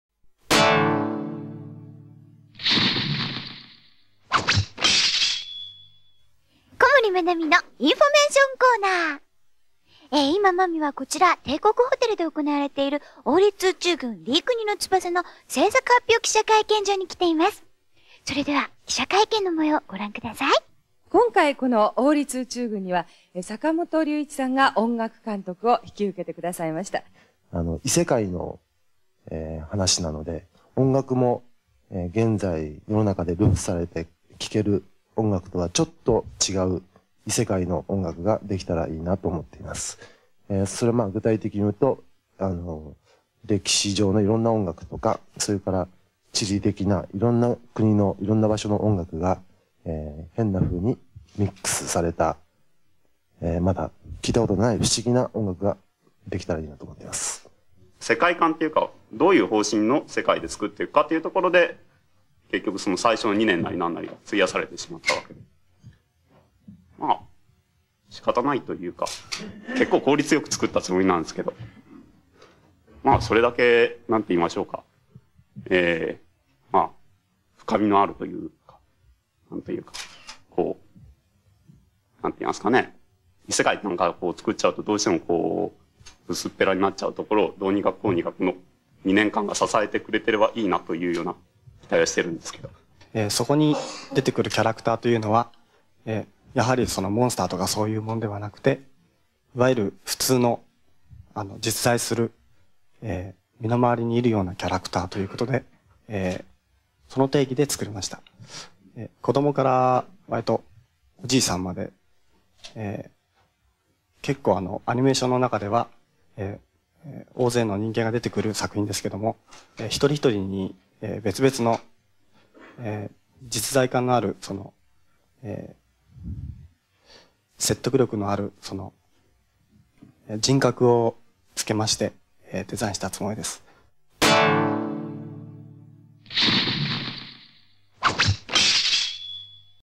王立宇宙軍 リイクニの翼 製作発表記者会見
製作発表当時はまだ『オネアミスの翼』ではなく、『リイクニの翼』という副題でした。